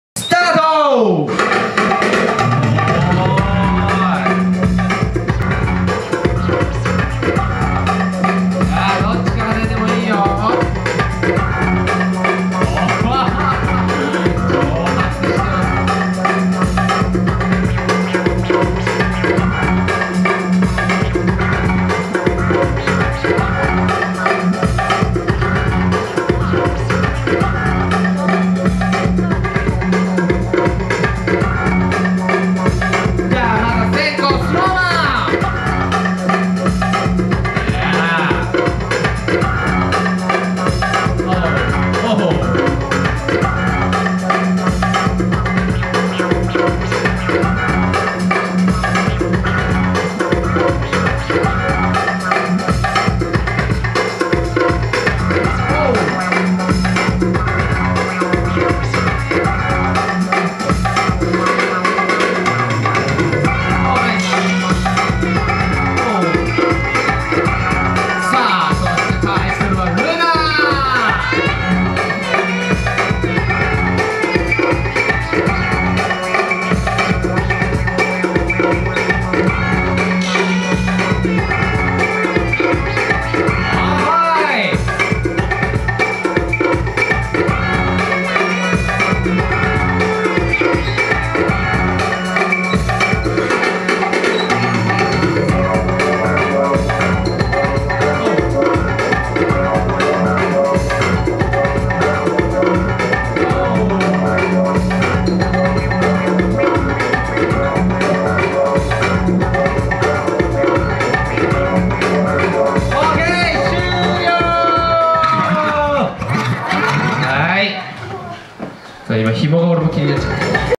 Kids 4回戦 RuNa(break) vs Snow Man(break) ~Dance Crystal~
2012/11/04 [Dance Crystal Vol.1] Free Style Solo Battle
Kids 総当り